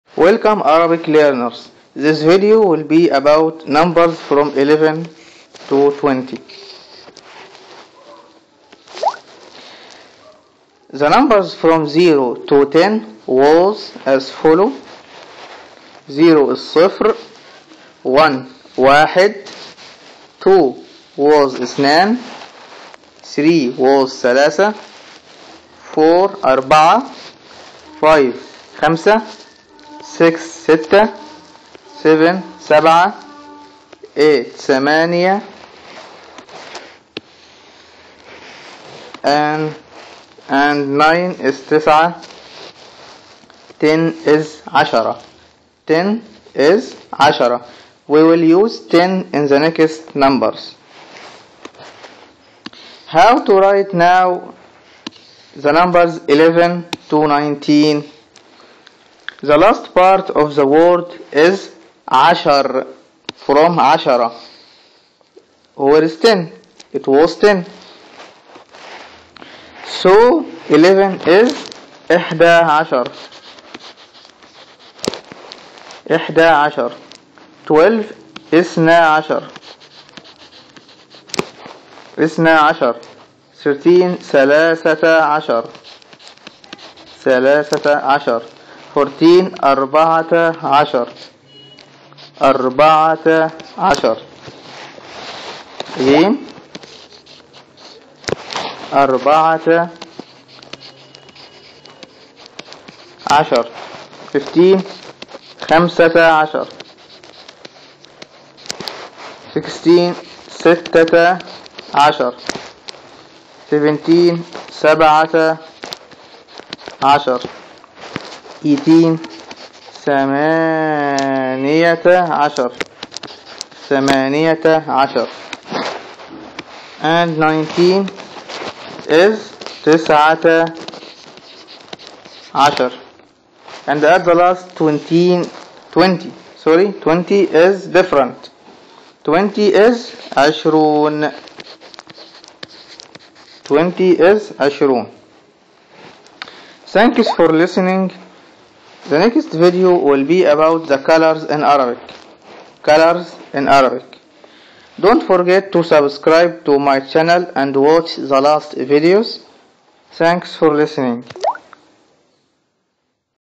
Numbers from 11 to 20: Learn Arabic #14
This video is about the numbers from 11 to 20, Don't forget to watch the last videos.